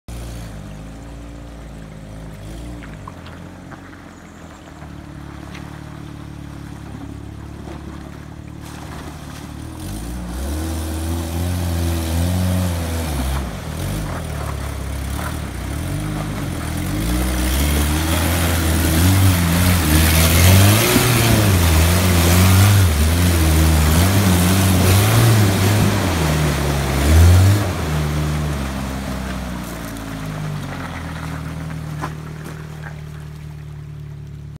Mud Runs in the Tracker (Clark Main)
Mud Runs in the Tracker (Clark Main) Grandpa Stearing the wheel.